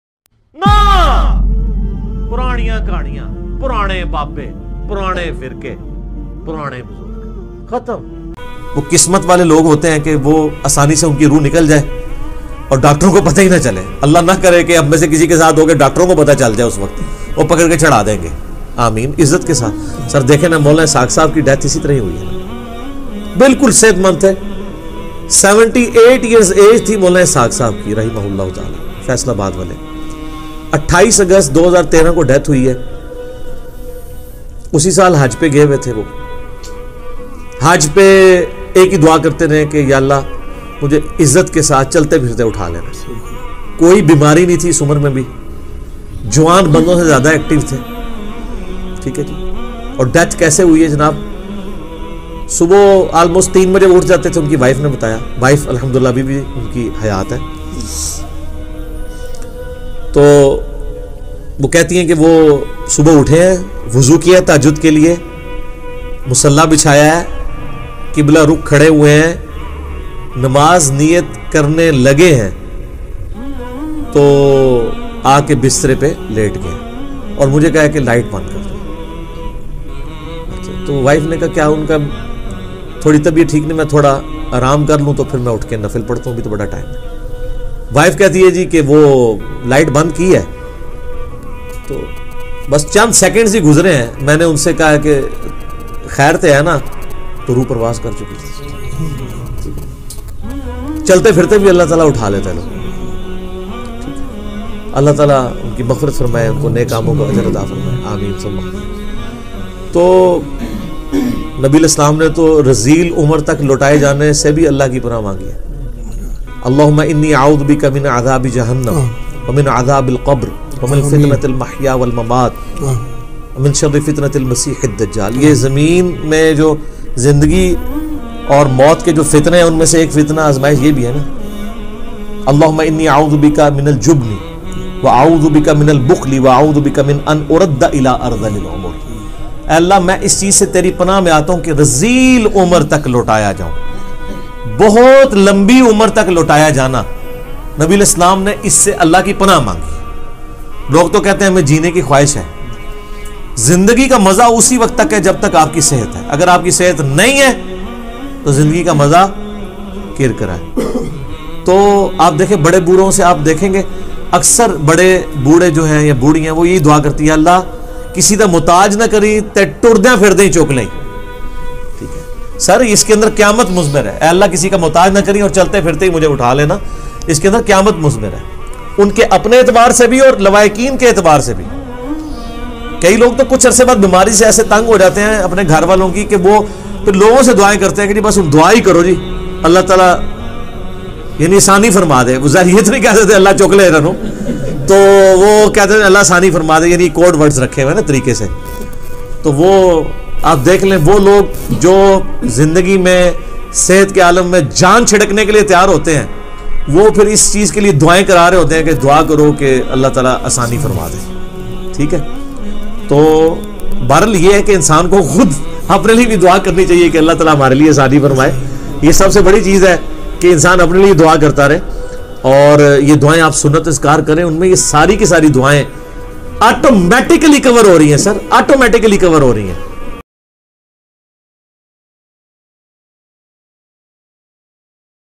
Behtareen mout @EngineerMuhammadAliMirzaClips
Title: Behtareen mout
In this video Engineer Muhammad Ali Mirza talking about the death of his ustad molana ishaq sahab.